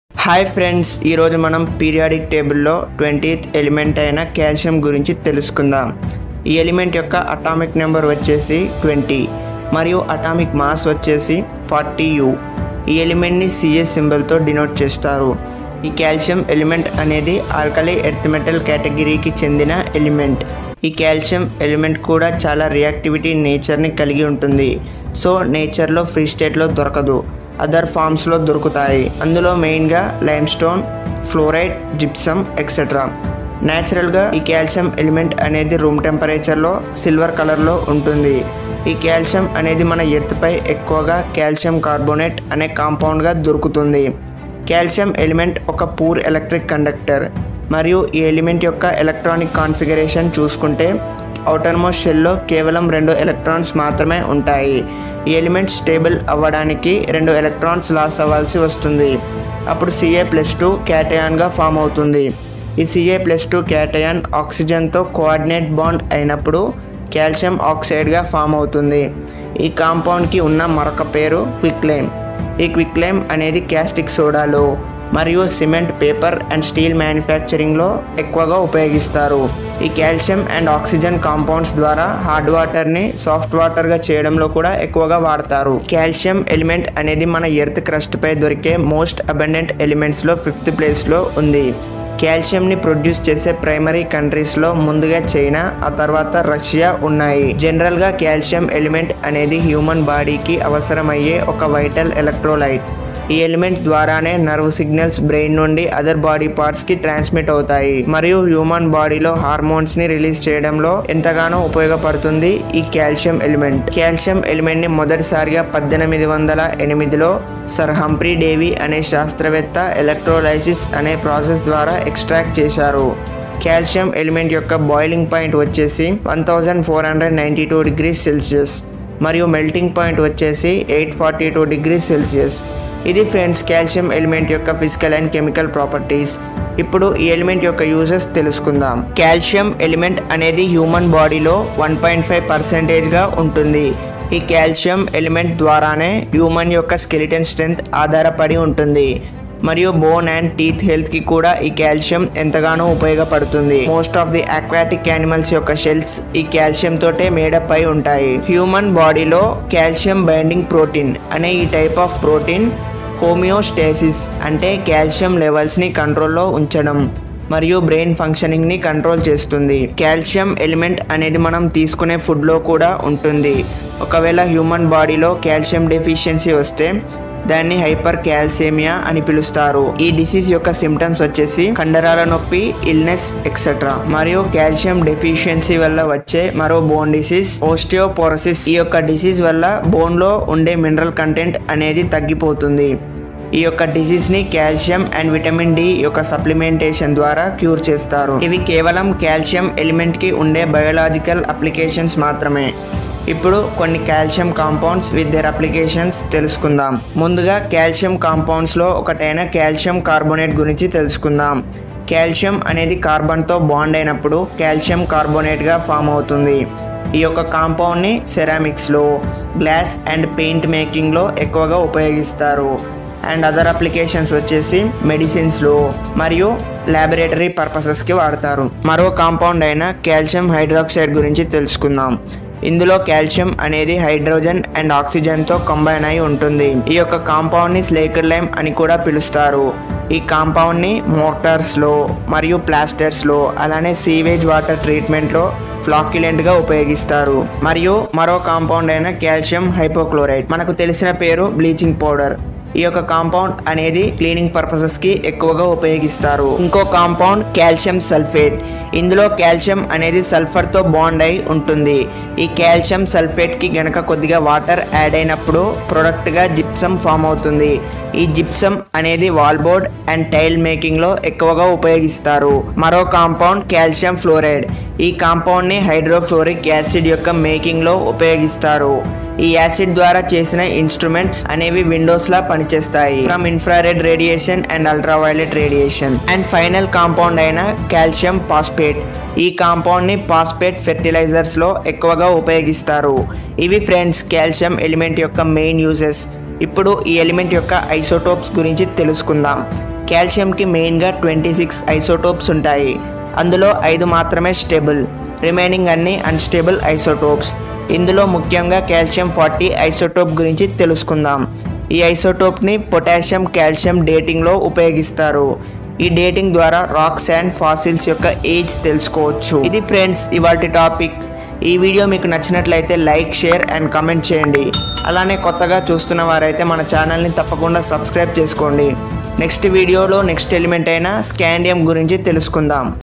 Calcium properties in Telugu|by Telugu knowledge|periodic table
Calcium properties in telugu by telugu knowledge

Today we will discuss about 20th element in periodic table Potassium physical,chemical properties in telugu and some important uses of Calcium In the series of periodic table this our 20th element Calcium Please